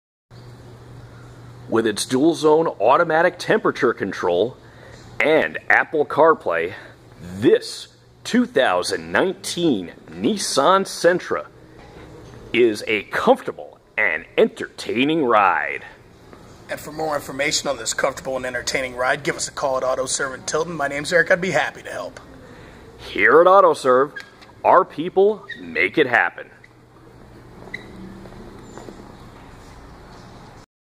2019 Nissan Sentra
With its Automatic Dual Temperature Control and Apple CarPlay, this 2019 Nissan Sentra is a comfortable and entertaining ride!